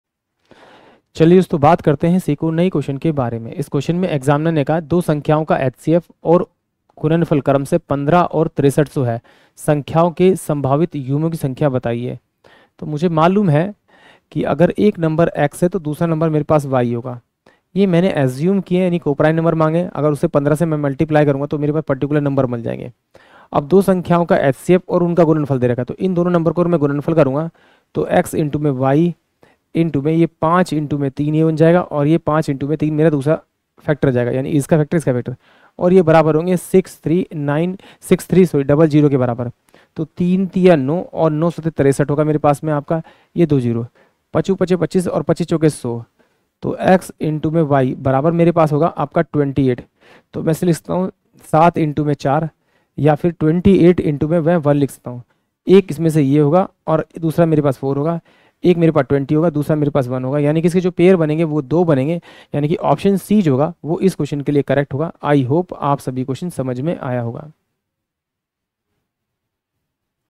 The HCF and product of two numbers are 15 and 6300 respectively. The number of possible pai
The HCF and product of two numbers are 15 and 6300 respectively. The number of possible pairs of the numbers is:
दो संख्याओं का HCF और गुणनफल क्रमशः 15 और 6300 है। संख्याओं के संभावित युग्मों की संख्या है:
4
3
2
1